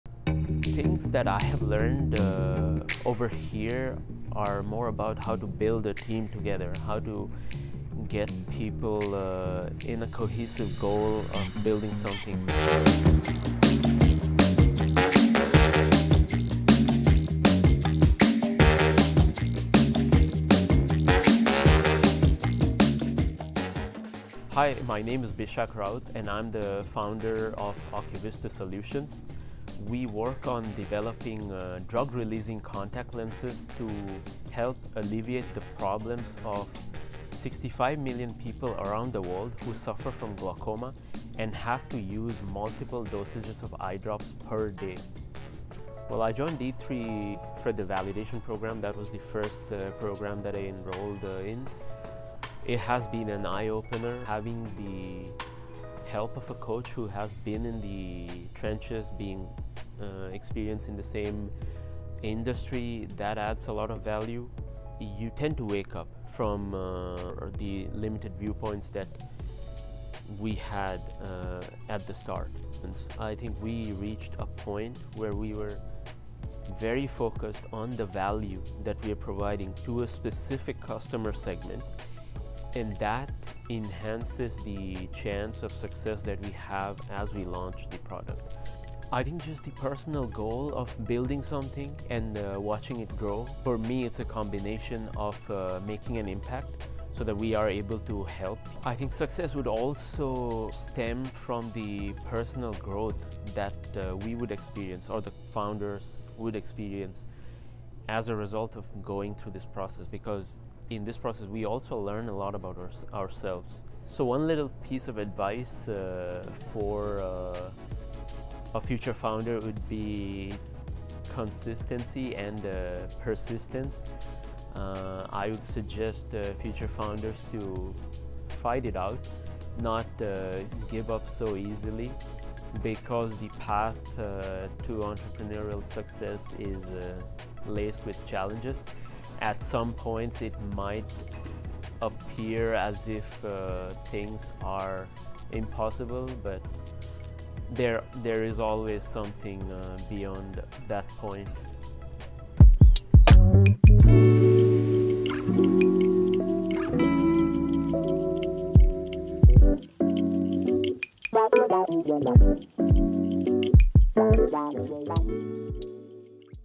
OcuVista Solutions - Founder Series
OcuVista Solutions: Part of District 3's Validation Program Spring 2024

Bishakh Rout, inspired by the desire to help people overcome limitations in healthcare, has developed a groundbreaking contact lens that delivers medication directly to the eye. This innovative technology has the potential to positively impact the lives of over 65 million people globally who suffer from glaucoma.

The Problem:
Delivering drugs to the eye presents significant challenges. Eye drops and lotions are cumbersome, leading to issues with patient adherence to treatment routines. Additionally, current methods of drug delivery to the eye often result in inaccurate dosing, leading to ineffective treatment of ocular diseases.

The Solution:
OcuVista offers a novel drug delivery system utilizing contact lenses. These lenses are comfortable, non-invasive, and encourage better adherence to treatment routines. With OcuVista's technology, drugs are delivered in a controlled manner, with sustained release over 8-16 hours, ensuring more effective treatment.

Social Perspective:
OcuVista is not just about better treatment; it’s about improving quality of life. By simplifying the delivery of essential medications and ensuring more accurate dosing, OcuVista empowers patients to take control of their eye health. The result is more effective treatment, better patient outcomes, and a potential reduction in the global burden of ocular diseases.

Latest News:
OcuVista is expanding its innovation beyond healthcare. We're currently working on a new project in the makeup industry, applying the same surface modification techniques used in our contact lenses. Additionally, we are in negotiations with a major medical device company to assist with their product development. We're also open to partnering with other companies for R&D in surface modification.